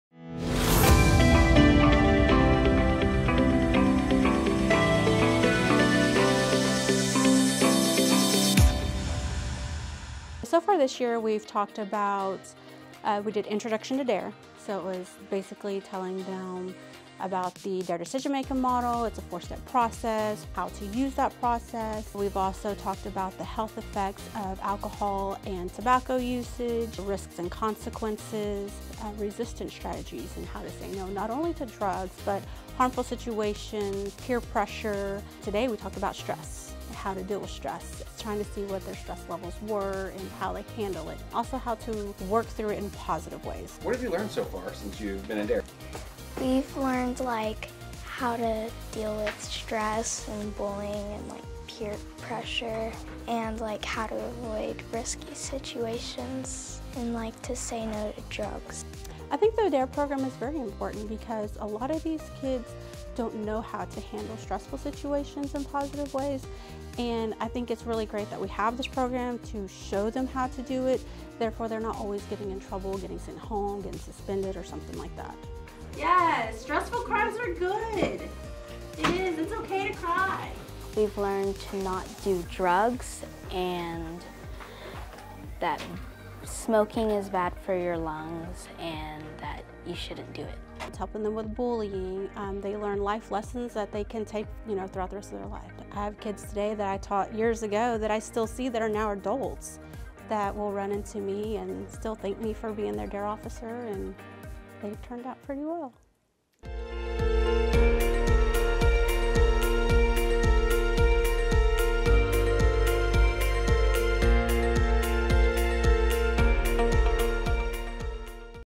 Lawton Public Schools: DARE Program
Students at Pioneer Park Elementary have been taking part in the DARE Program for the last few weeks and it's defiantly making an impact! Watch this video to see the amazing lessons they have learned. LawtonPS WeAreLPS